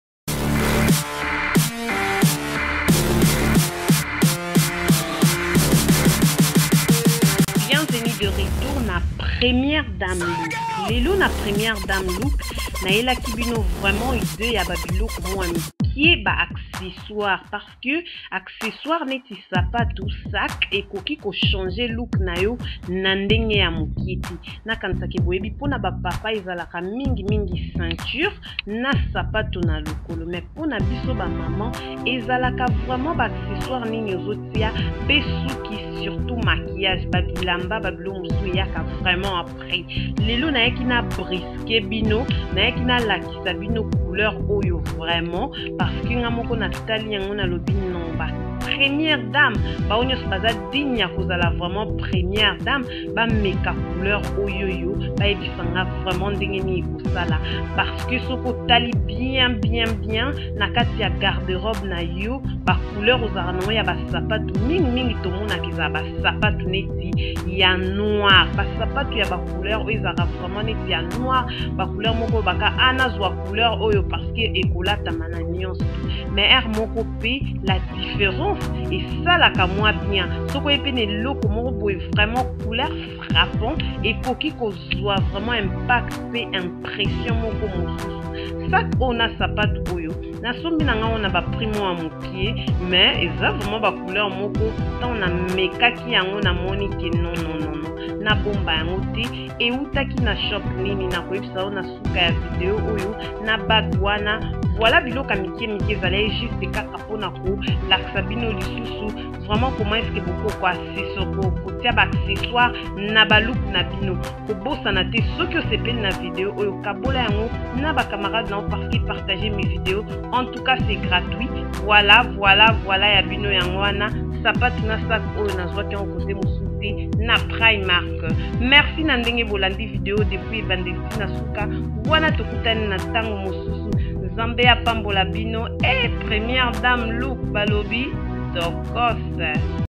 Lookbook #1 Ba accessoires ya COULEUR nini ezalaka bien pona ba maman ?
Bonjour, Mbote, Hello !! lelo Tokeyi nanu naba idee ya couleur nini eza bien pona ete? Idee ya lelo eza sapatu na sac ya couleur pona ko marquee difference.
« Si tu aimes mes vidéo, partager c'est gratuit » ❤❤❤